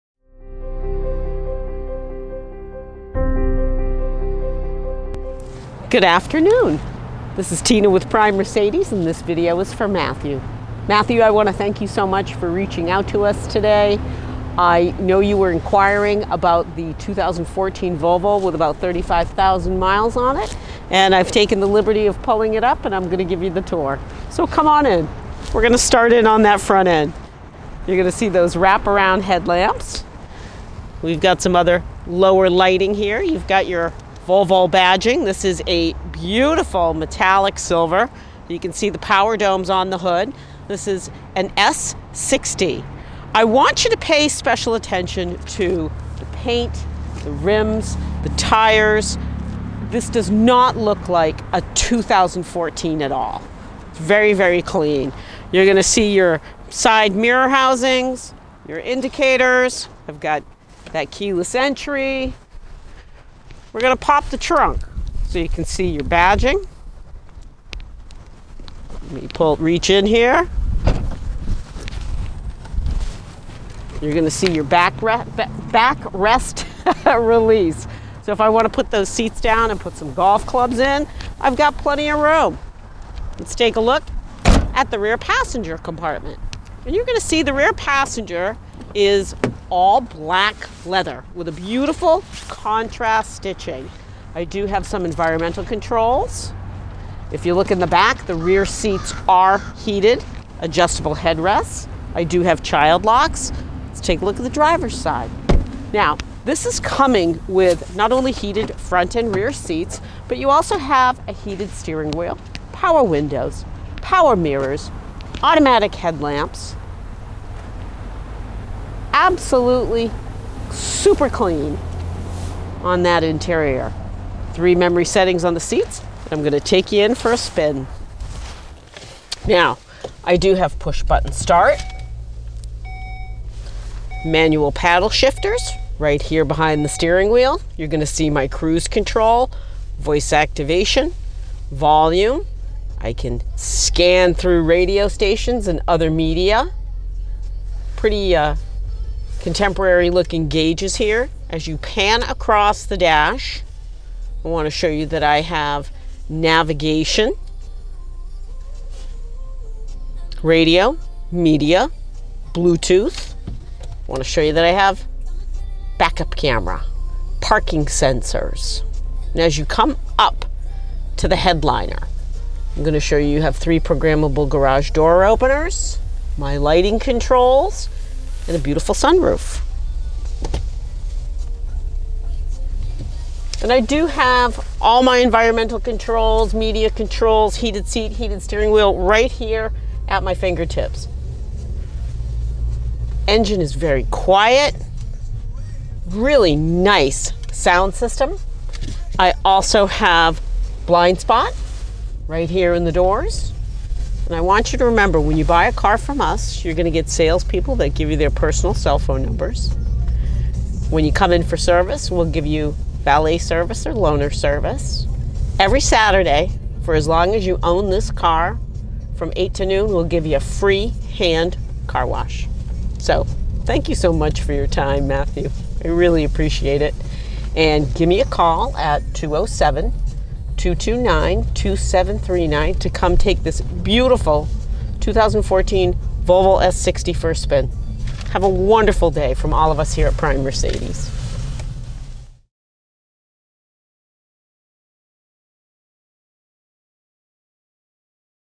2014 Volvo S60 T5 Platinum video tour with Tina
Just traded here at Prime Motor Cars Mercedes Benz in Scarborough Maine. Loaded with leather, heated seats, navigation and back up camera. 1 owner, must be seen to be appreciated.